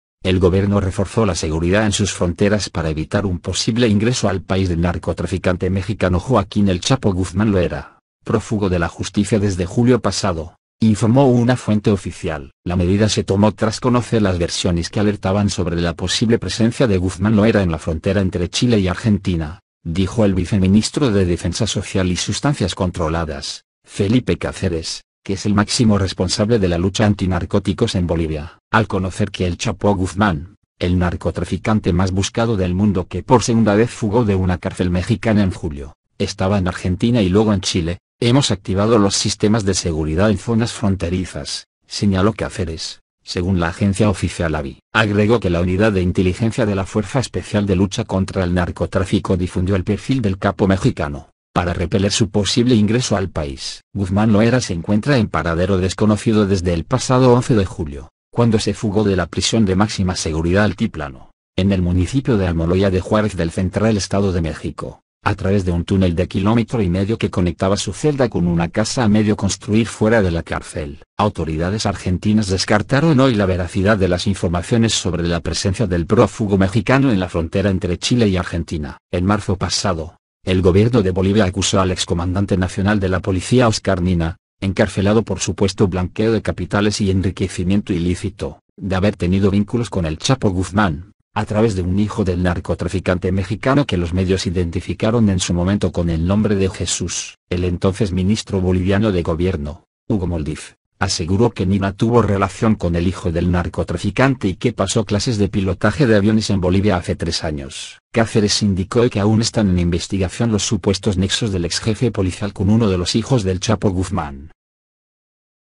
Bolivia refuerza seguridad fronteriza para evitar ingreso de Chapo Guzmán
El Gobierno reforzó la seguridad en sus fronteras para evitar un posible ingreso al país del narcotraficante mexicano Joaquín el "Chapo" Guzmán Loera, prófugo de la justicia desde julio pasado, informó una fuente oficial.